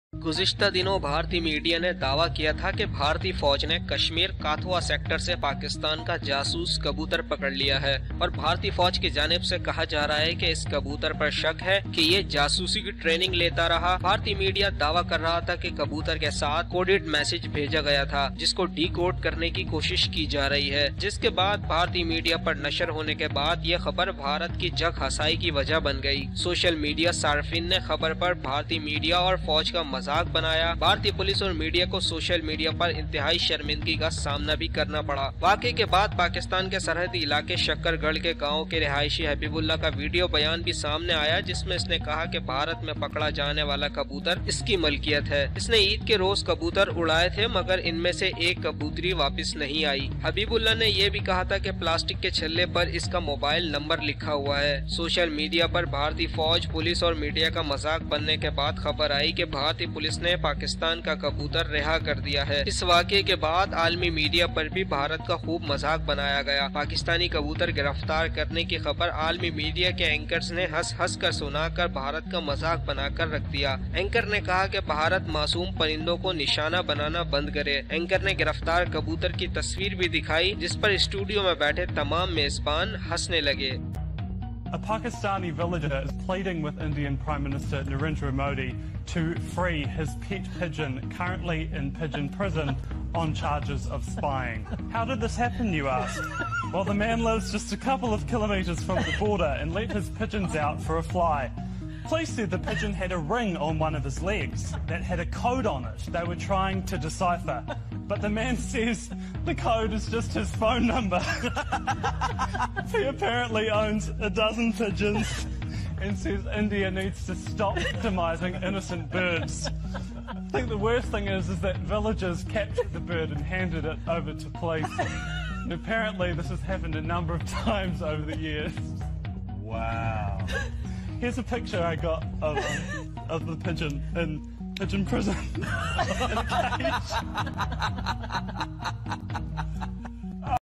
International media funny reaction on Pakistani spy pigeon captured by India [[[[////Current News///
In this video the we bring the funny reaction of International media  of India Blaming on Pakistan